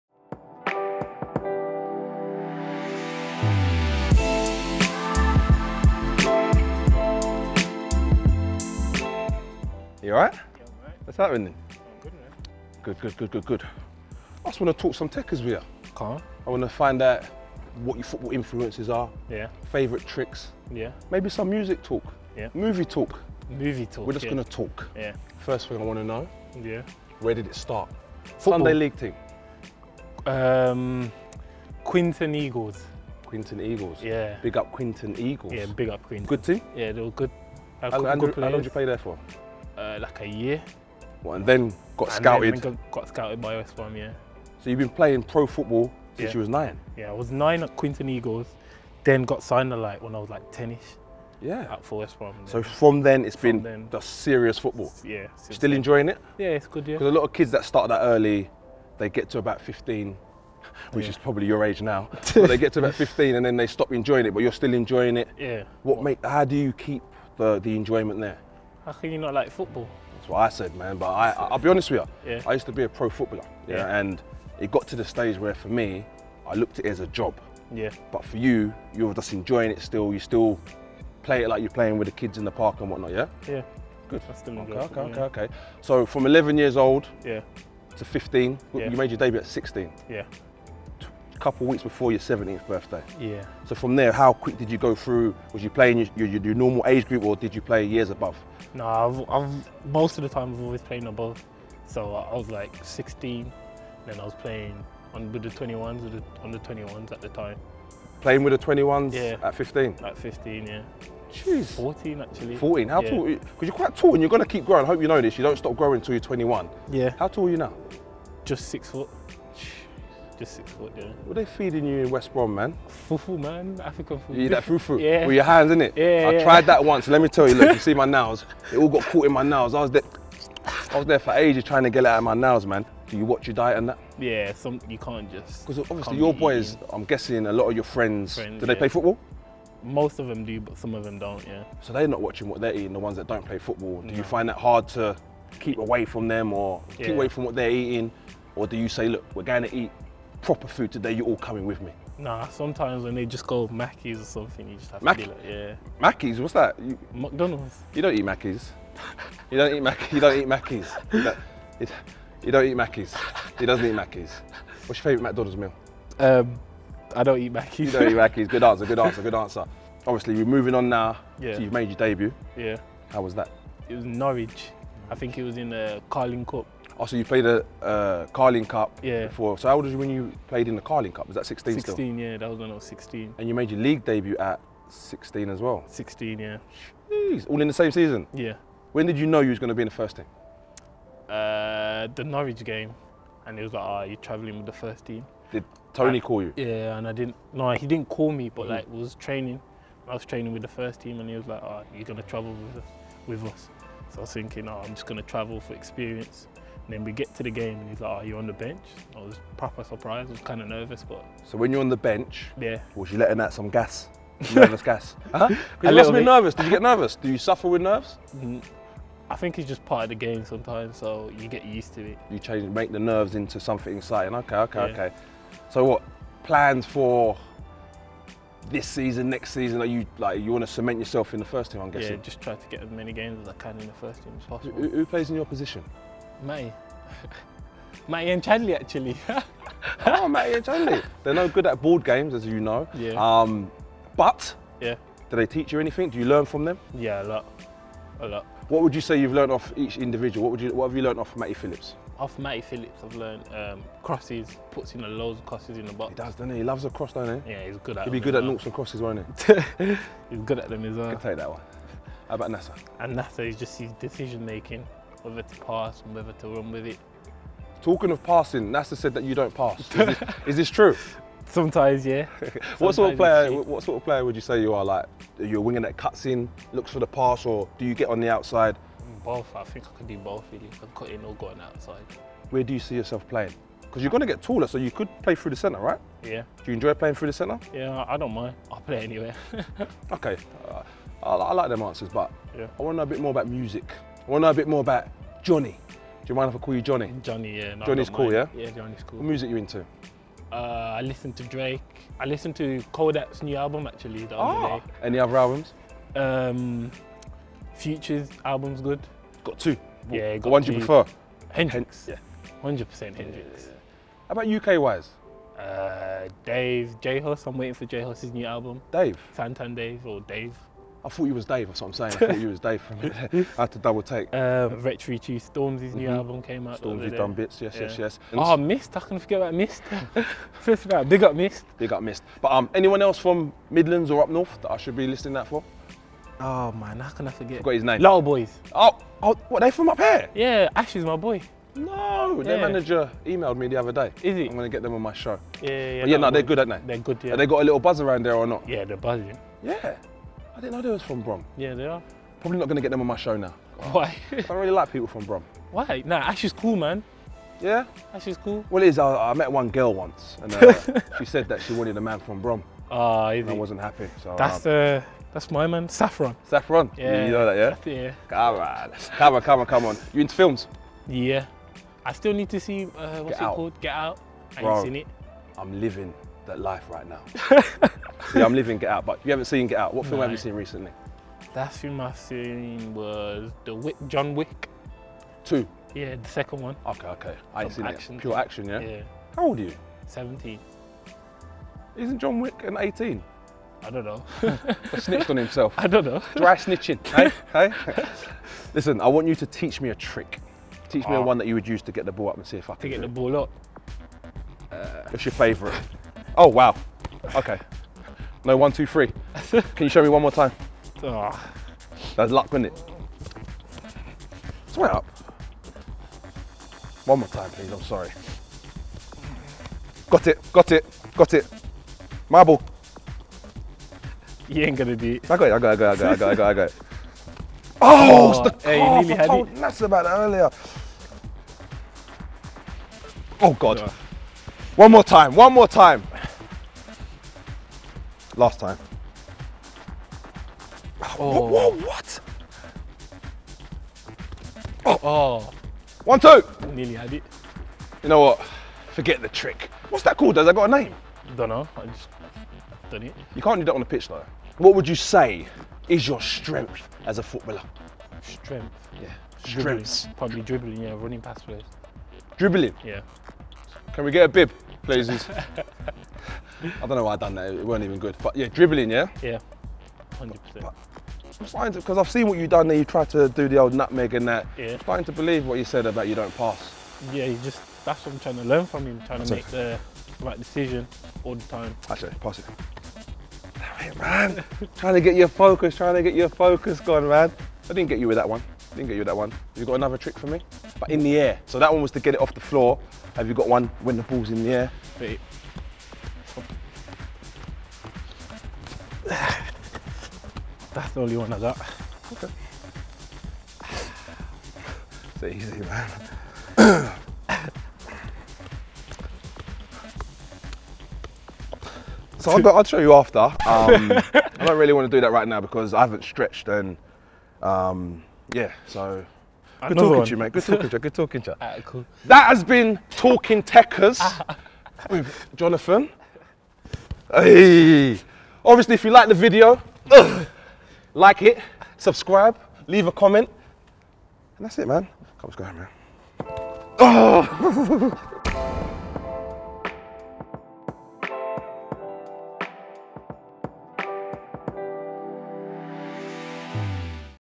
Jonathan Leko of West Bromwich Albion TalkingTekkers With Stevo The Madman
The second episode of our new series in which JD Football meet with Europe's most exciting young ballers, West Bromwich Albion's Jonathan Leko joins Stevo The Madman for TalkingTekkers. The England Under 17 international breaks down his journey so far, his music tastes and more...